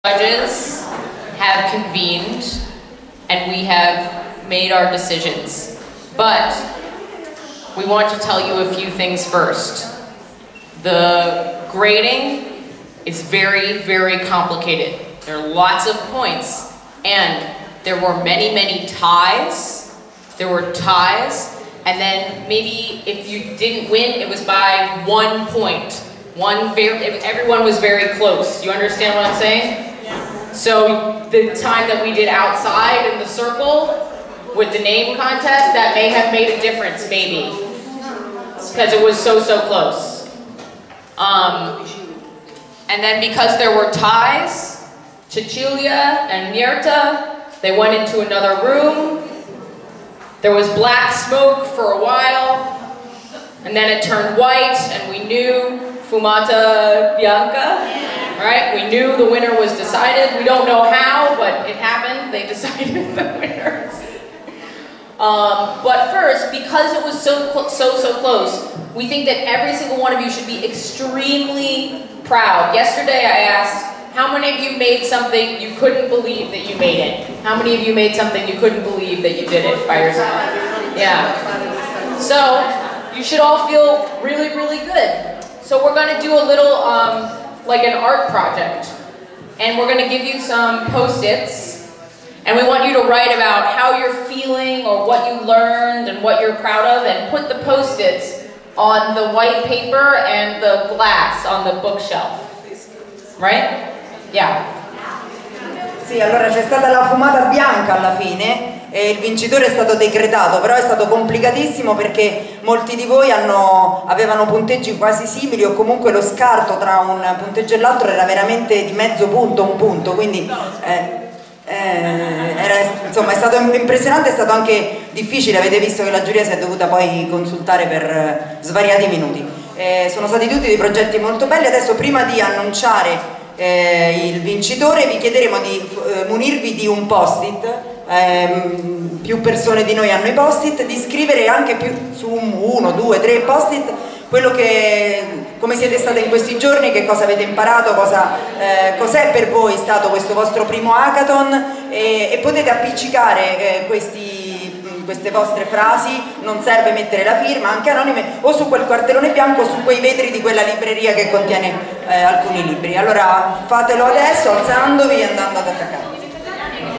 CodingGirls Roma-Usa: "Ti lascio un messaggio"
L'hackathon è terminato e le ragazze aspettano la "fumata bianca"... cioè prima dell'annuncio delle squadre vincitrici. Ma prima lo staff invita tutte le ragazze a lasciare un breve messaggio in bacheca, con un post it giallo.